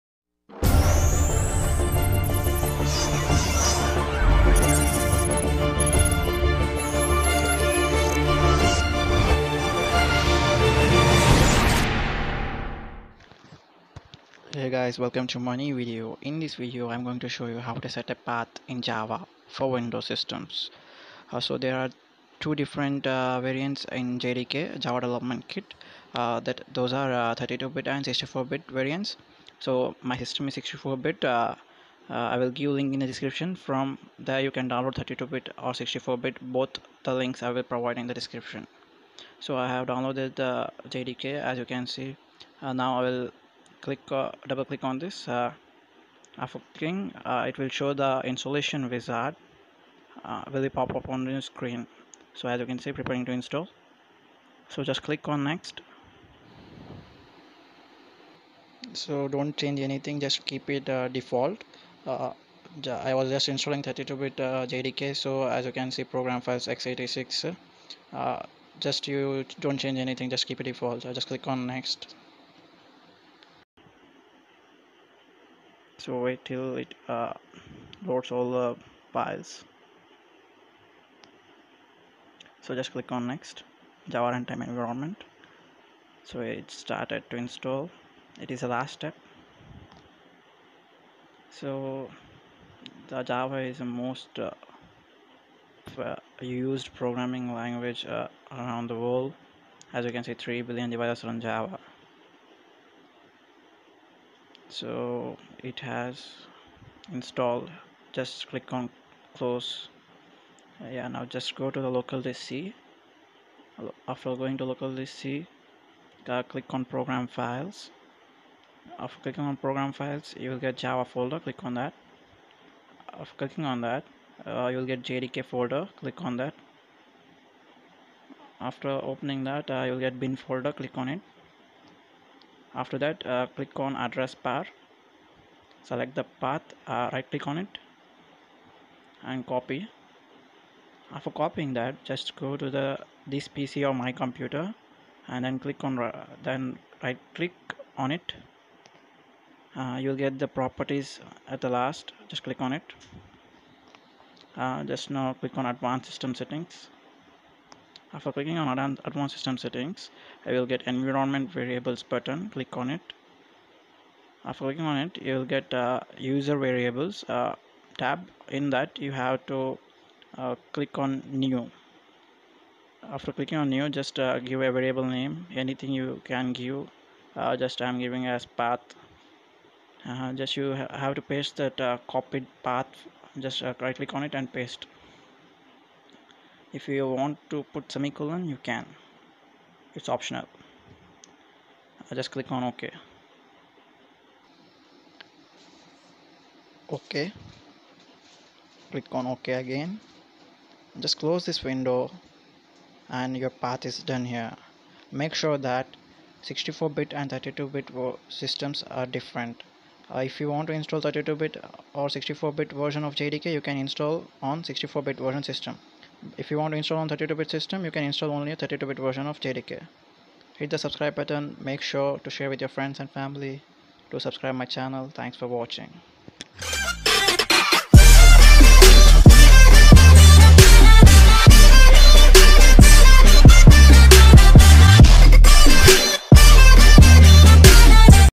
How to set a java path in windows System 32 bit
32 bit Windows Systems - Download 32 Bit Version JDK

64 Bit Windows System - you can download any JDK i.e, 32 Bit or 64 Bit version